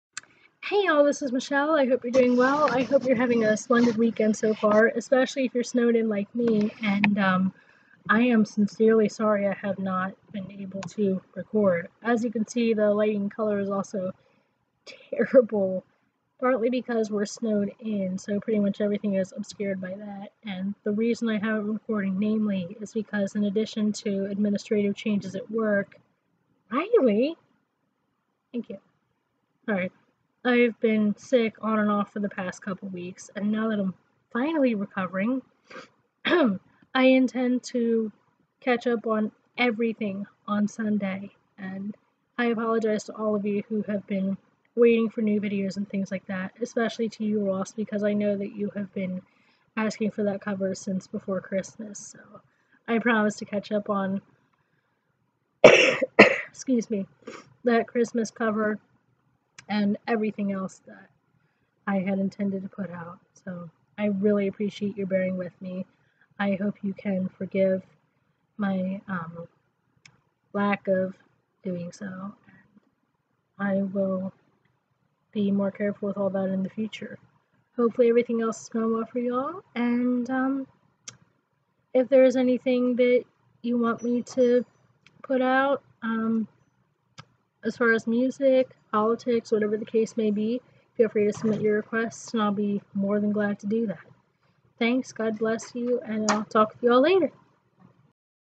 Updates and Apologies!
To all who were waiting for the cover I promised over Christmas, I apologize and hope you can forgive me! I have had major administrative changes at work in the past month in addition to illness and being snowed in today! I will catch up on everything I said I would tomorrow when I am recovered completely and will resume normal operations then. Thank you so much for bearing with me, especially to my most faithful subscribers and viewers!